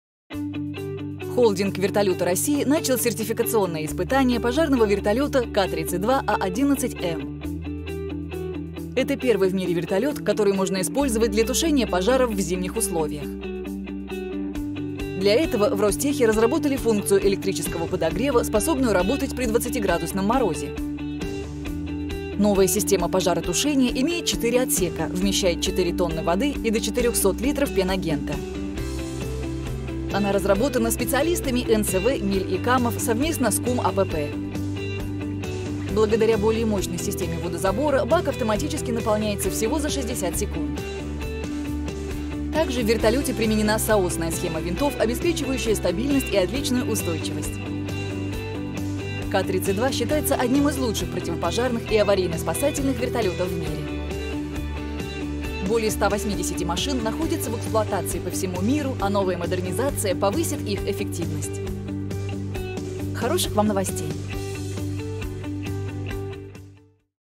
Новейший пожарный вертолет Ка-32А11М проходит испытания
Первый в мире вертолет, который может тушить пожары даже при 20-градусных морозах. Ростех приступил к сертификационным испытаниям модернизированного Ка-32А11М